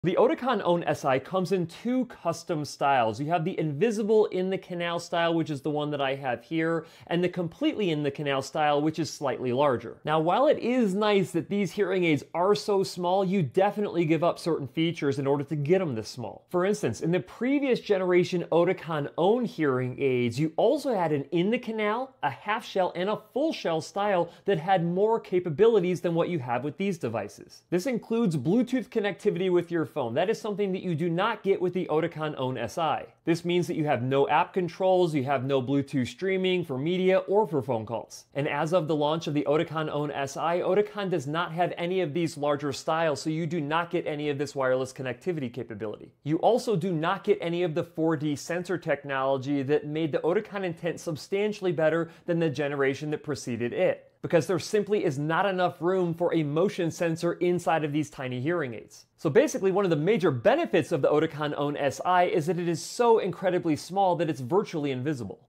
Invisible Hearing Aids: What Do You Lose?
These new hearing aids from Oticon are TINY, and basically invisible. However, that small size comes with some real trade-offs.

What do you think...worth it?